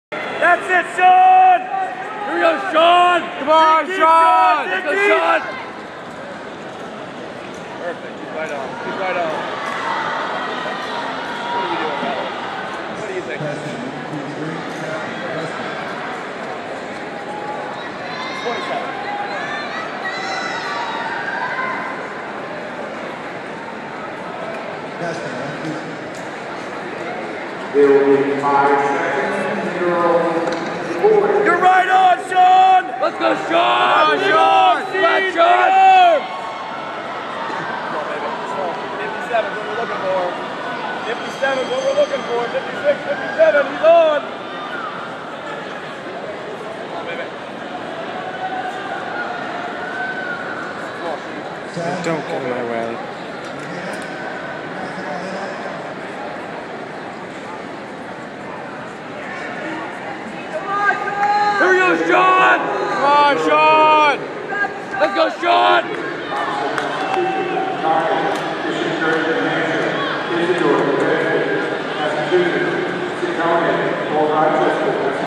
Sean MacDonald at New England
Sean running the 600 at New England Saturday. Sean got a PR in his race. Sean is very good. Great job yesterday Sean.